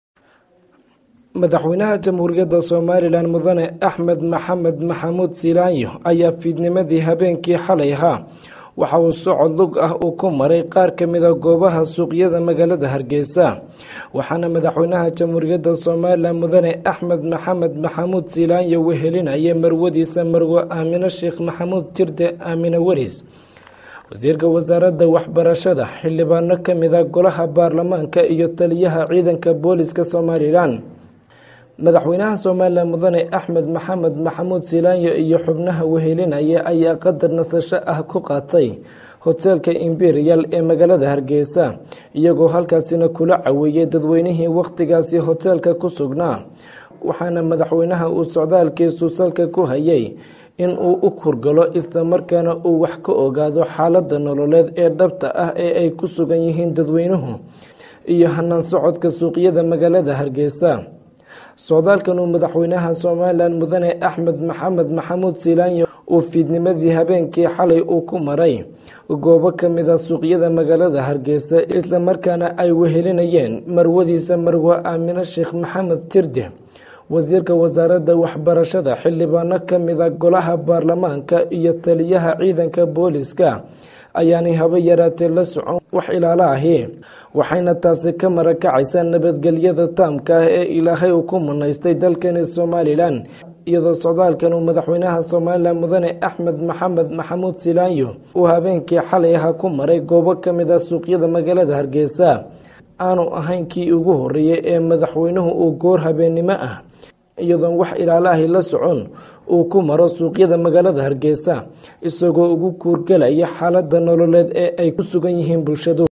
Madaxwaynaha Somaliland Oo Dhexlugeeyay Suuqa Caasimada Somaliland Isagoona wadan Wax Ilaali Ah
Socdaalkii Kadiska Ahaa Ee Uu Madaxweyne Siilaanyo Xalay Kumaray Qaar Kamid ah Suuqyada Magaalada Hargaysa